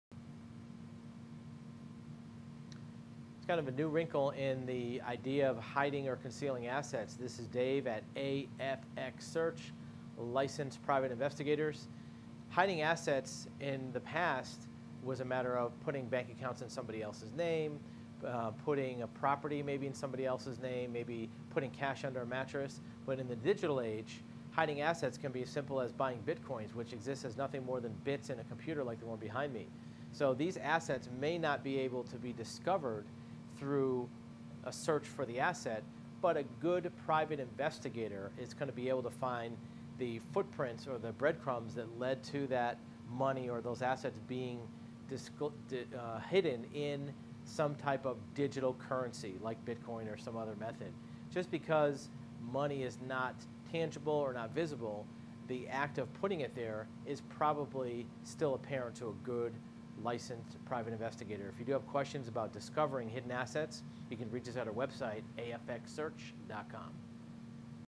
Investigating hidden or concealed assets in the digital age | AFX
Hiding assets in the past was a matter of putting bank accounts into someone else's name, putting property in someone else's name, hiding money under a mattress. In this video we will discuss what asset recovery looks like in the digital age.